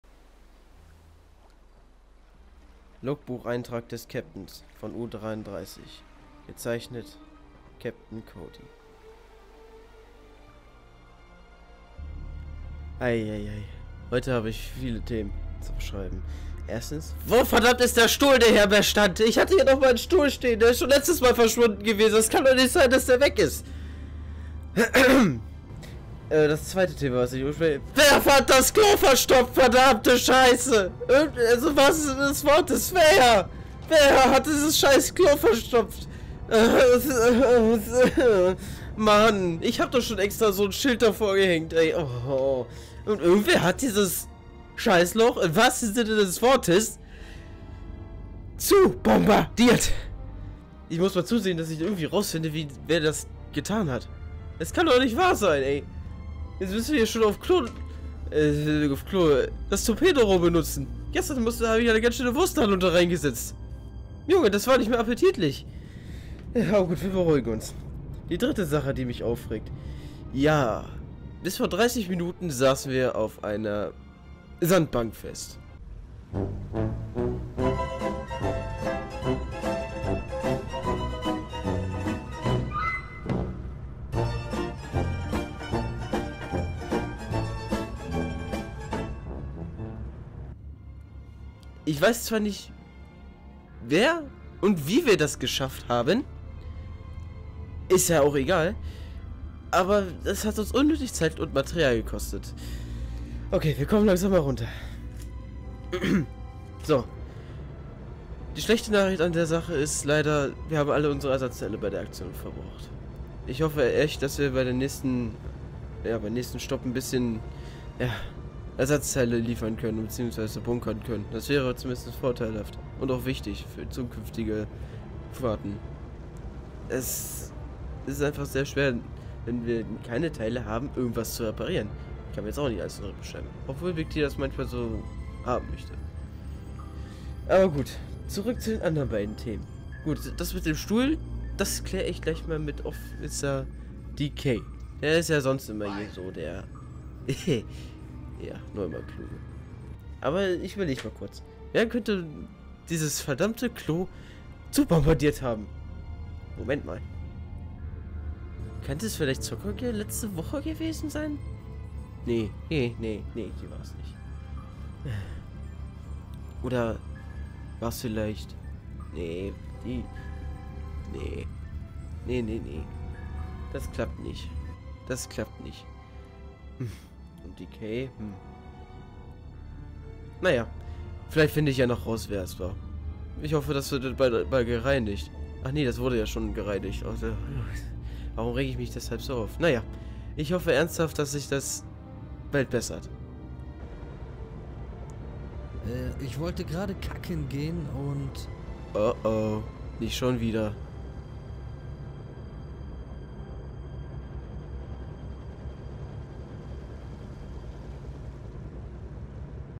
Die Besatzung Kapitän Codys Logbuch #2
Es ist wieder soweit. Kapitän cody öffnet das logbuch von U33. Mal schauen was es neues an Bord von U33 gibt. Und vorallem welche "Probleme" es auf U33 gibt.